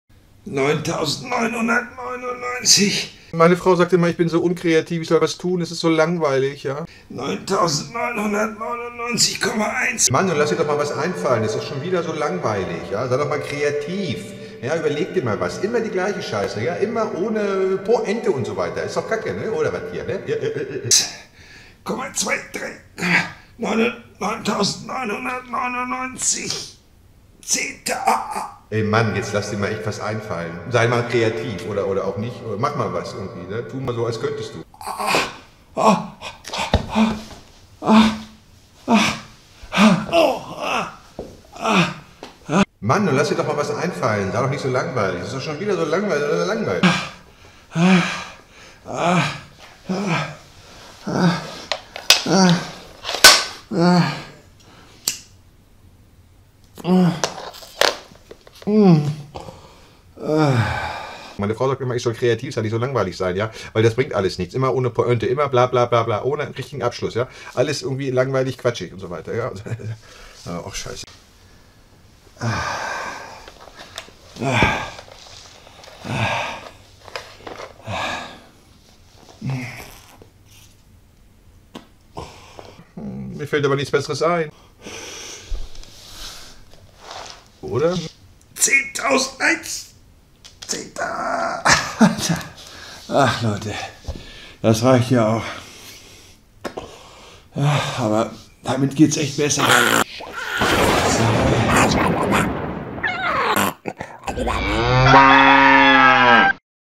Meine Frau sagt: Das ist doch alles langweilig (ohne Po-Ente) | Theo
Meine Frau sagt: Das ist doch alles langweilig (ohne Po-Ente).
Und sie hat recht! Liegestütz und Rauchen, mehr fällt mir nicht ein.
Hahaha, tolle Nummer...